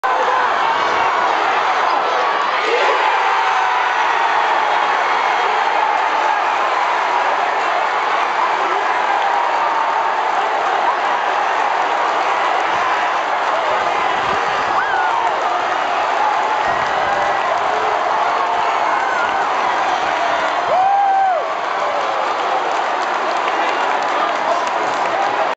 Dublin v Kerry: Kevin McManamon goal
Goal scored by Dublin's Kevin McManamon, All-Ireland SFC semi-final, 1/9/2013.